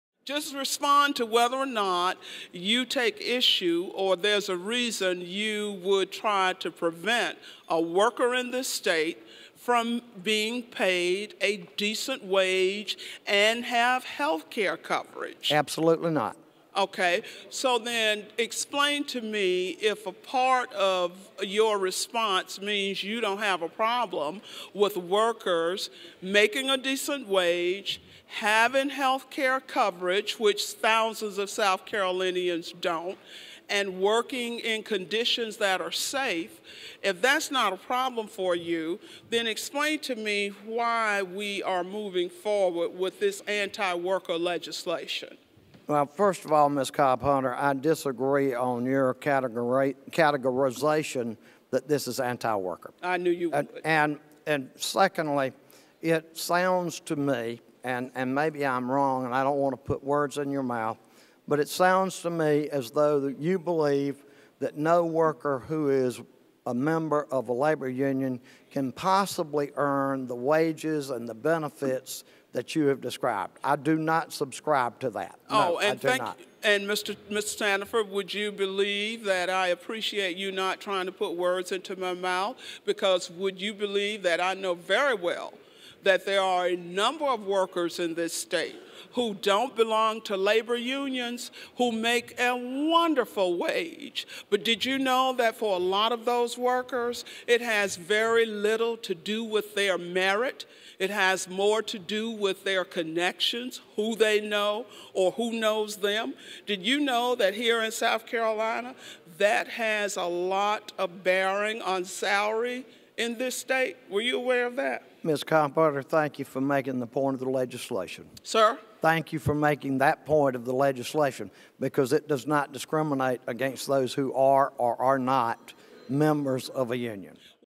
The House on the Right to Work Bill
On Wednesday, February 8th 2012, the House debated H. 4652, known as the Right to Work Bill. In this clip Representatives Gilda Cobb-Hunter and Bill Sandifer discussed Labor Unions and the Project Labor Agreement (PLA).